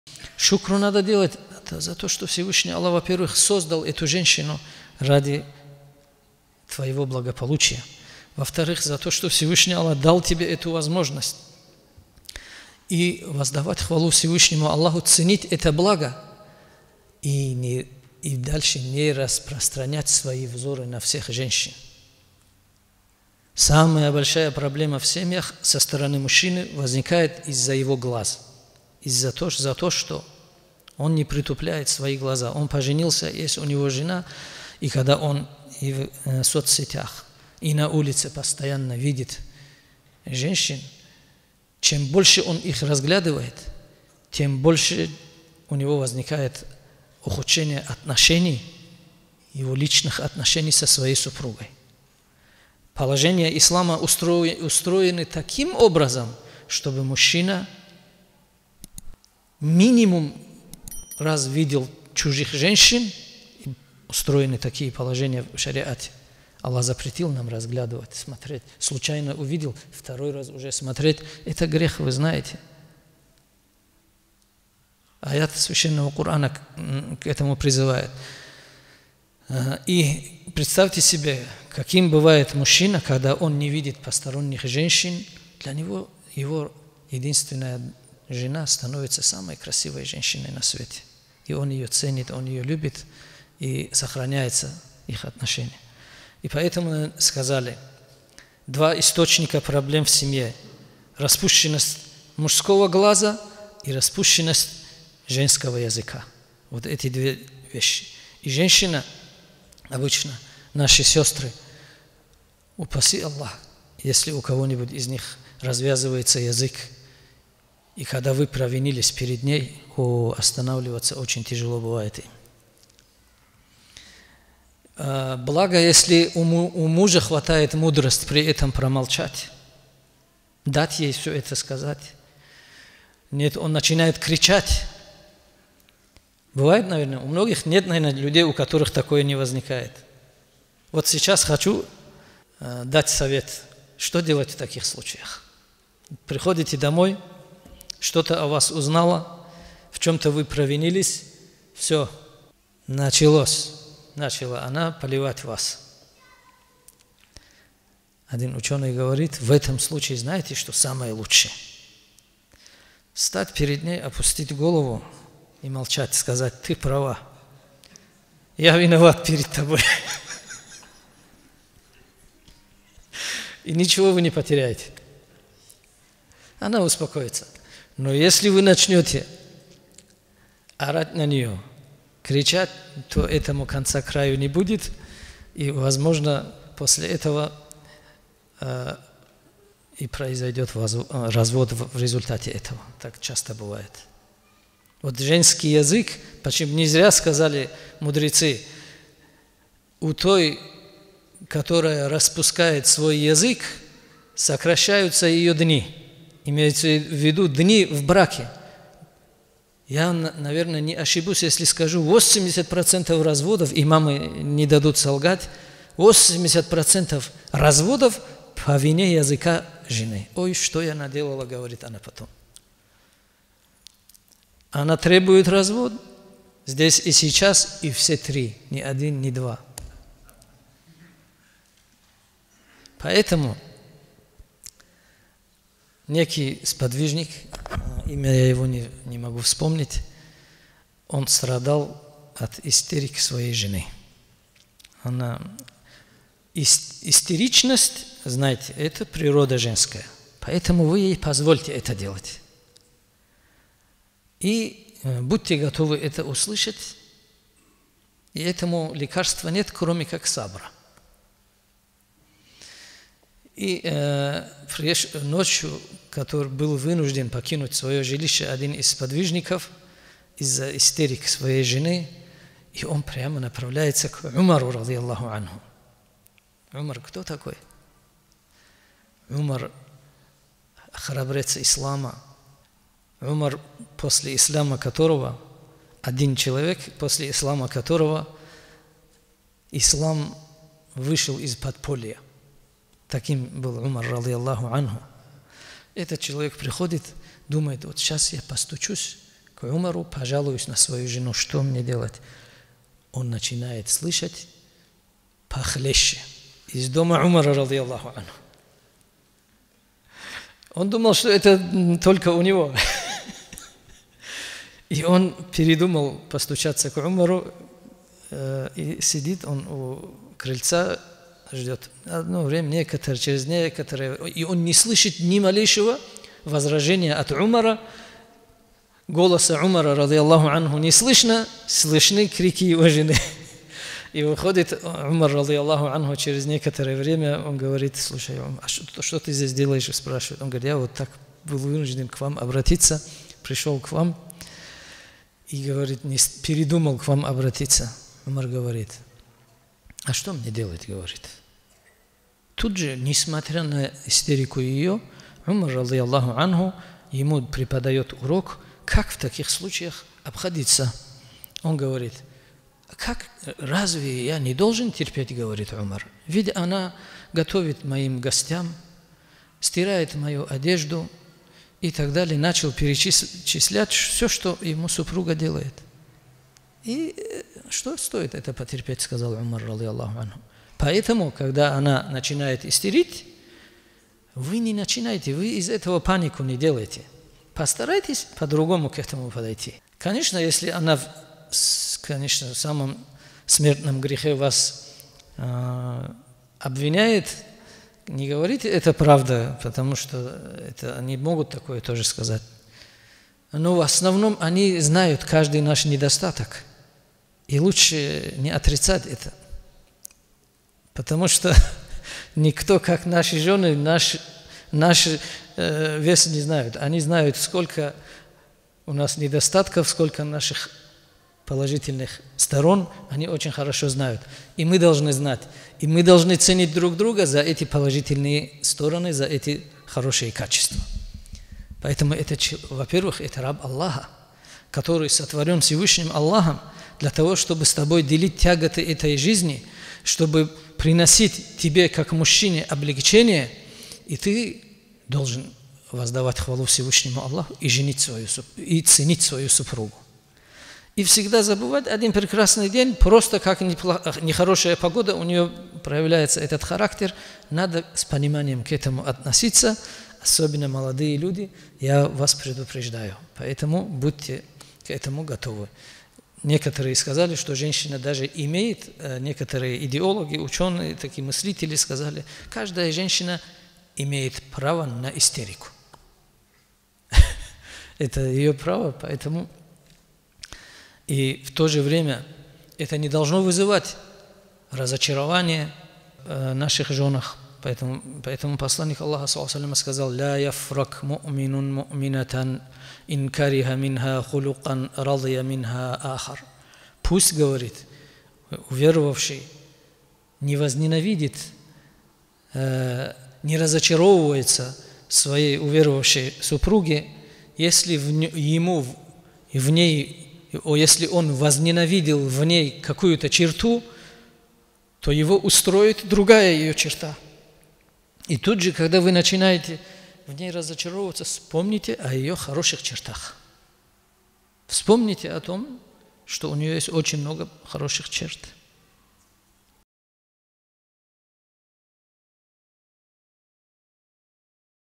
Взоры на женщин и истерика жены / Наставление семьям / Саадуев М-Расул
Отрывок проповеди в джума-мечети г. Избербаш на тему способов сохранения семьи.